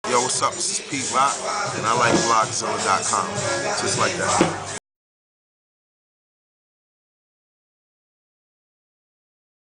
Pete Rock BlogXilla Drop
Pete Rock At In Store For New Album in NYC